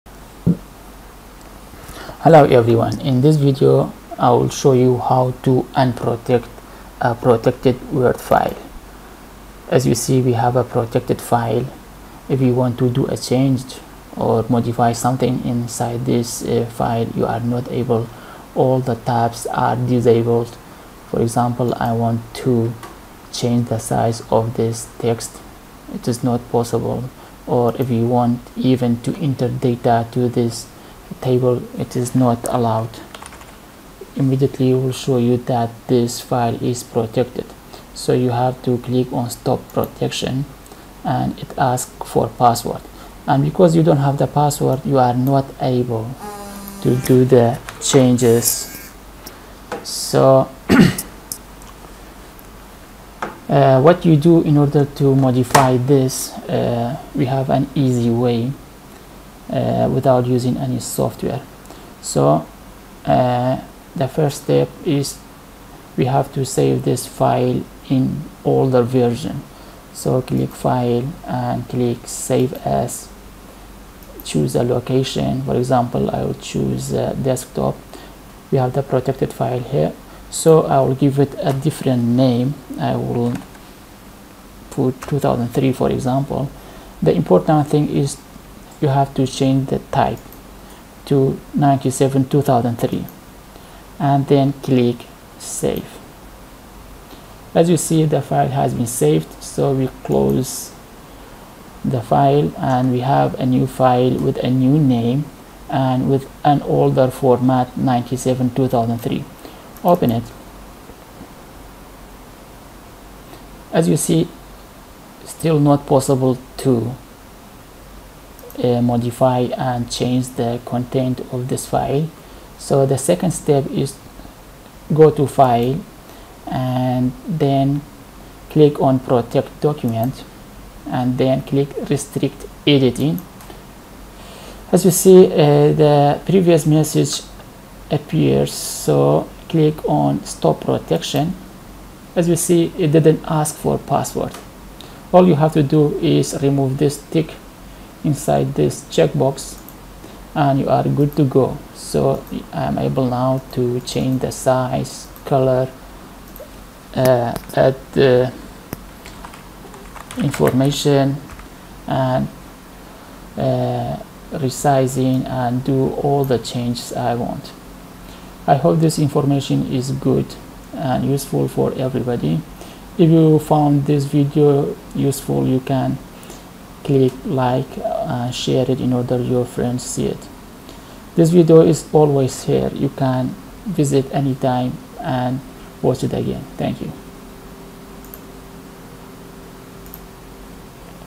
How to Edit/Modify Protected word file
in this video, I will show you how to modify and edit a protected word file without using any software, the earset way is shown in the video